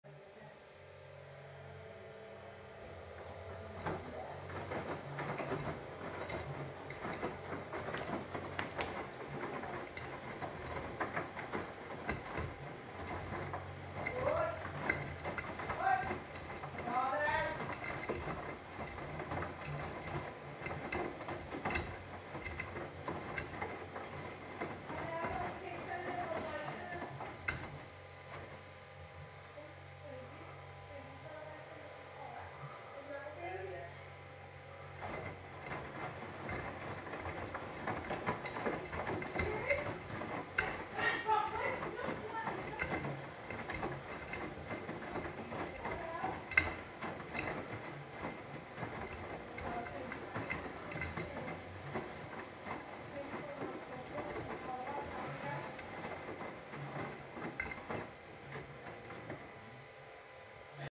Beko WTG921B3W - Cottons 90° Fast+ Heating Tumbles
Washing a small load of towels on Cottons 90° with Fast+ in this Beko. used a bold pod. Reached around 80° I think, door glass felt hotter than my old one ever got.